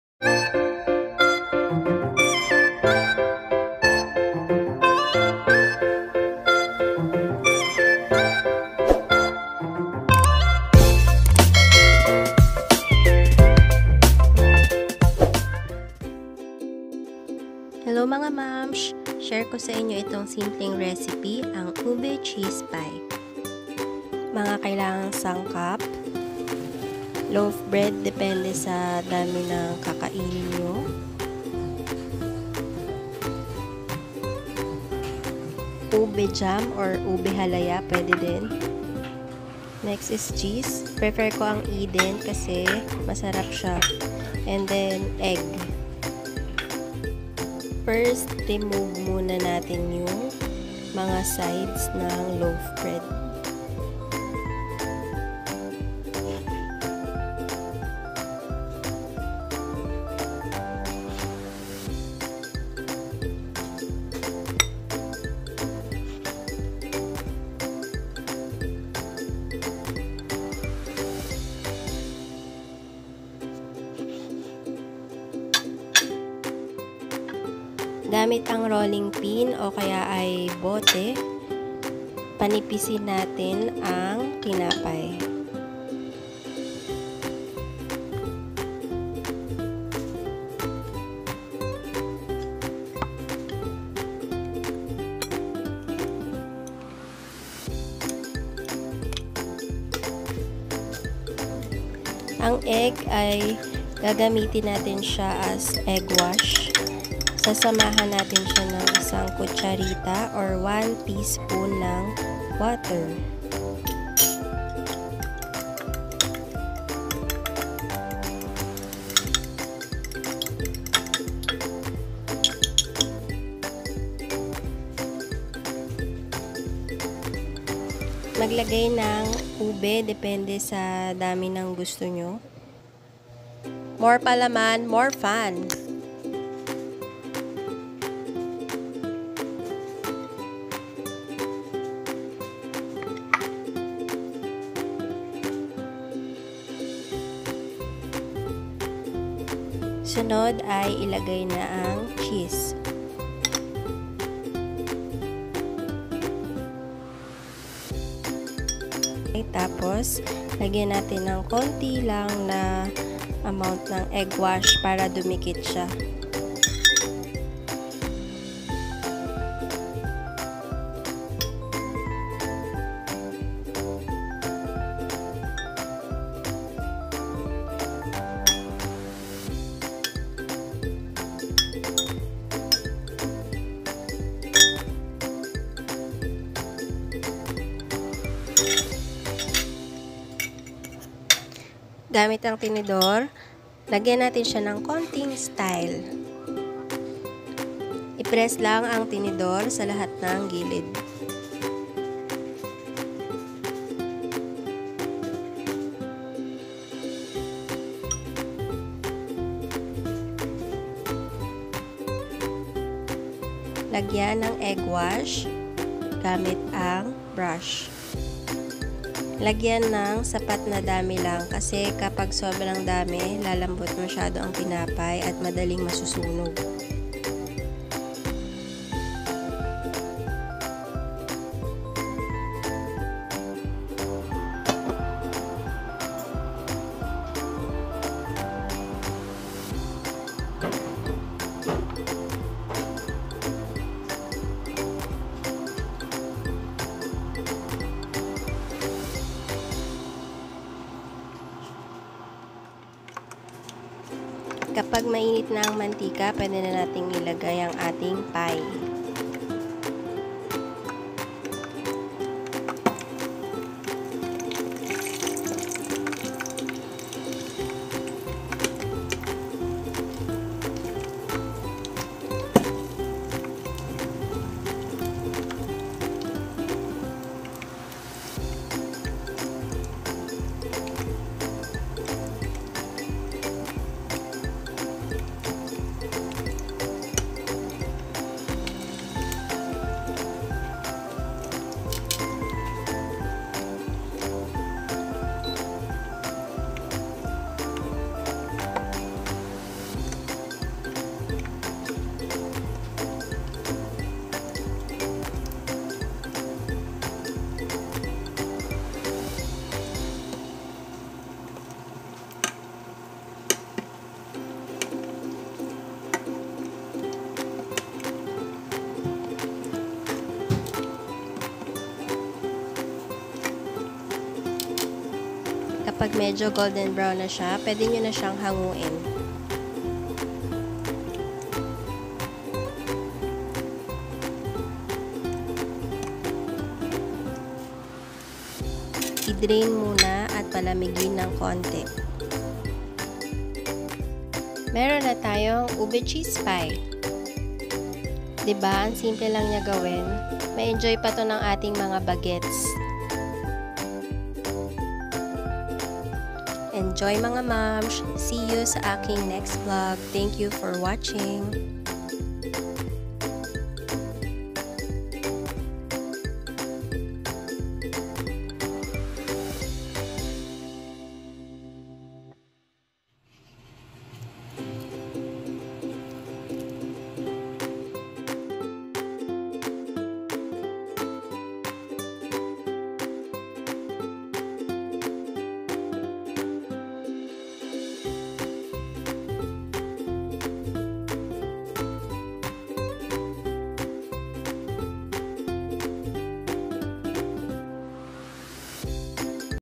UBE CHEESE PIE | JOLLIBEE INSPIRED | DIY RECIPE
UBE CHEESE PIE
INGREDIENTS:
LOAF
UBE HALAYA/ JAM
EGG
CHEESE
COOKING OIL
TYAGA AT PASENSYA

if you have your personal ways to do this, you can have it that way. 😊